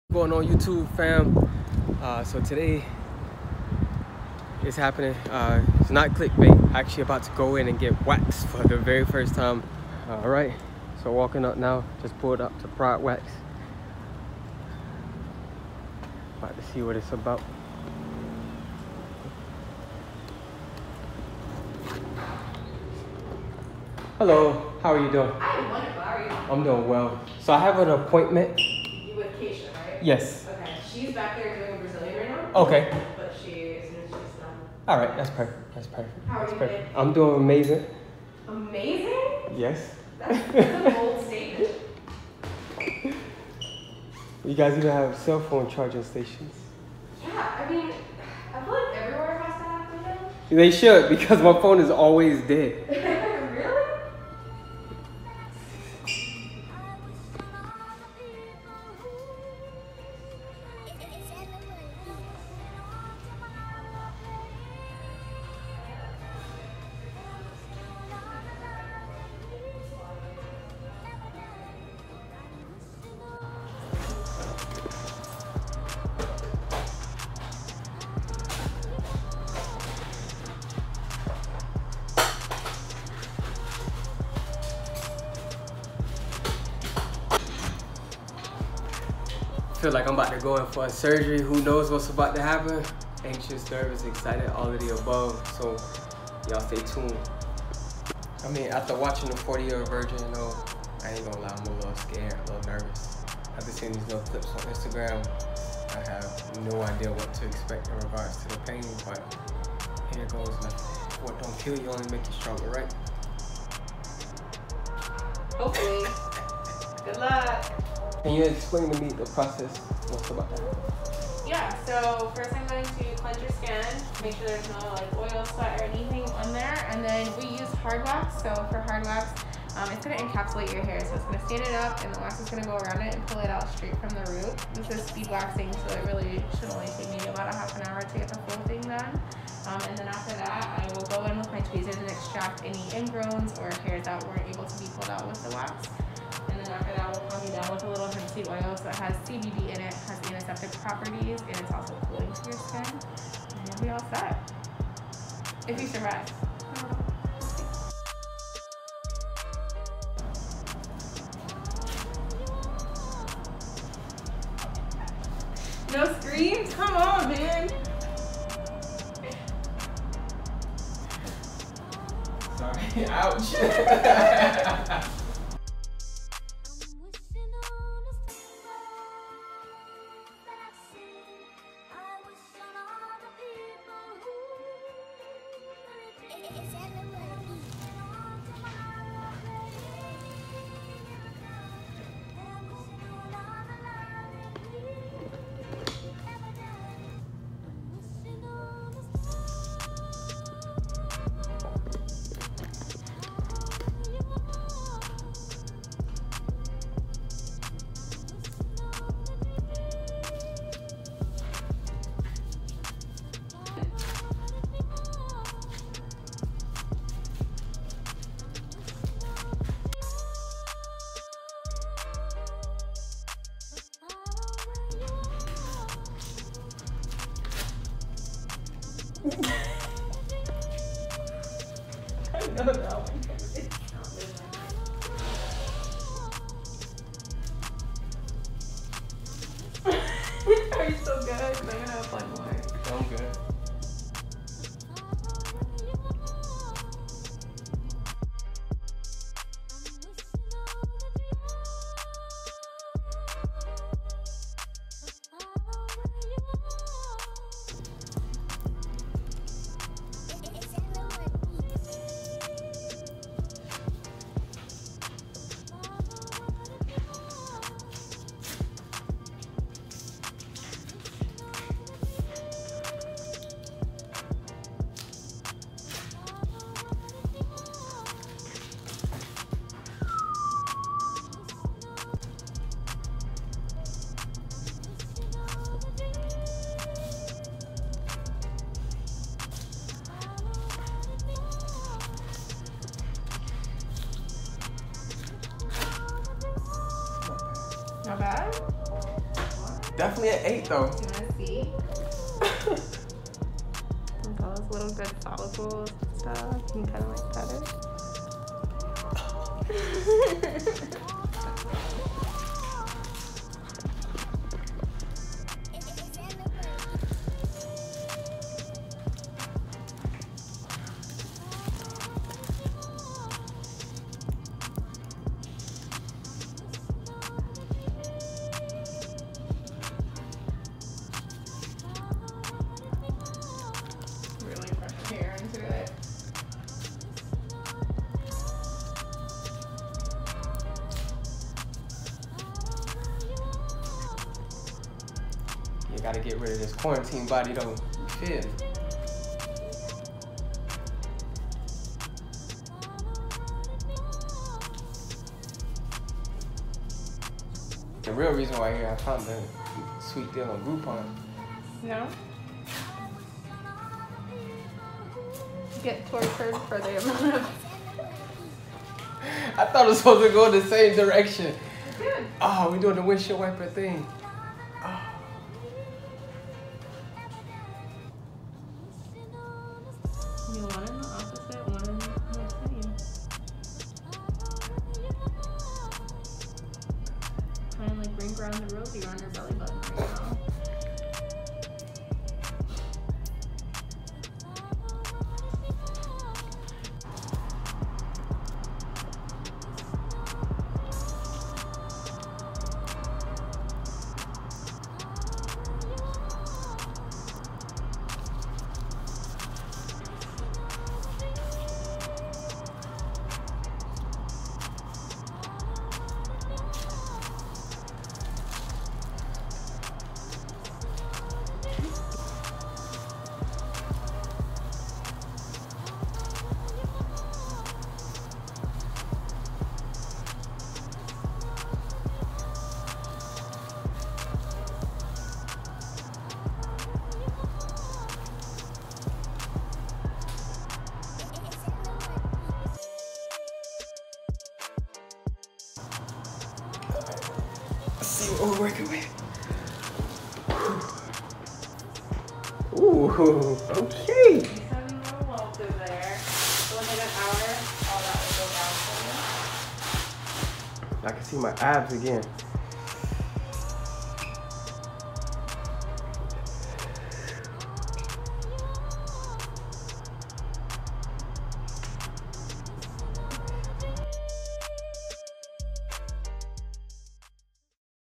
Getting Waxed For The First Time!
Decided to get rid of the quarantine body and get waxed for the 1st time.